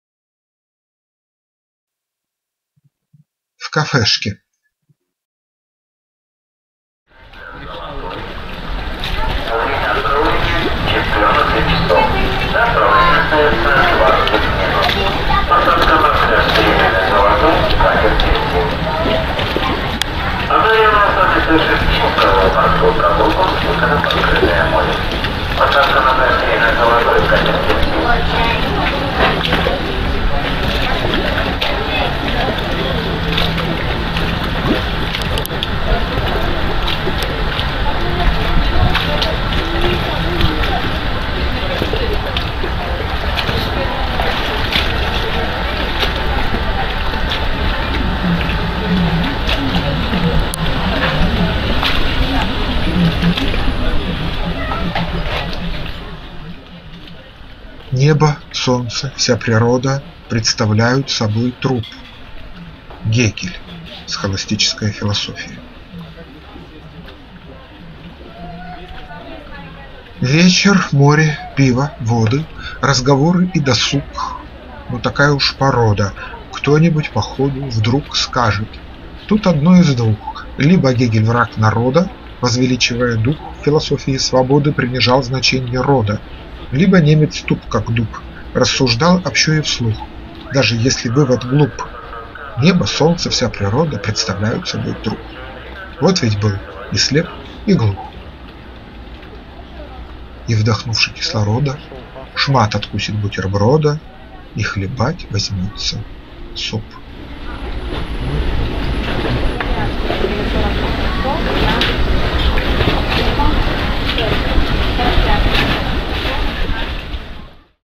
В кафешке
Олег Кустов читает стихотворение "В кафешке".

Стихотворение 2012 года из сборника "Метафизическое путешествие" (2013).

Небо, солнце, вся природа
представляют собой труп.
Г.В.Ф.Гегель. «Схоластическая философия»

Вечер, море, пиво, воды,
Разговоры и досуг.
Но такая уж порода –
Кто-нибудь по ходу, вдруг
Скажет:
              «Тут одно из двух!
Либо Гегель враг народа –
Возвеличивая дух
В философии свободы,
Принижал значенье рода.
Либо немец туп, как дуб, –
Рассуждал общо и вслух,
Даже если вывод глуп:
Представляют собой труп”.
Вот ведь был и слеп, и глух».

И вдохувши кислорода,
Шмат откусит бутерброда
И хлебать возьмётся суп.